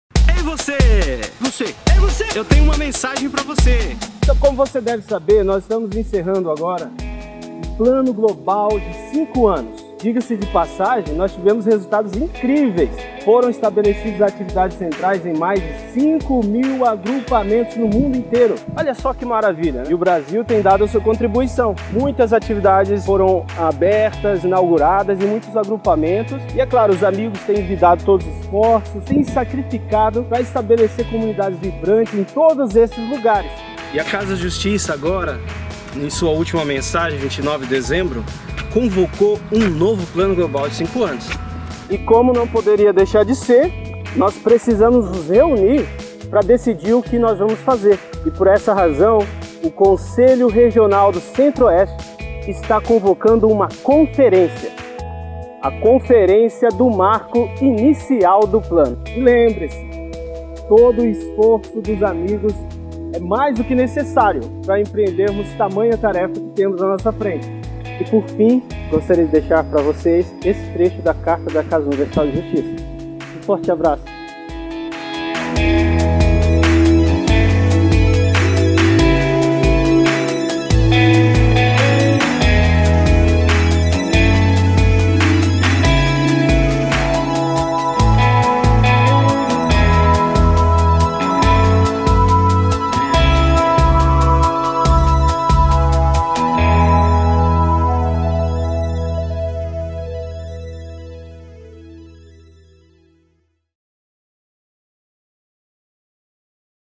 A Conferência do Marco Inicial do Plano
O Conselho Bahá'í do Centro -Oeste stá convocando uma conferência. :)

27 a 29 de maio em Gama-DF.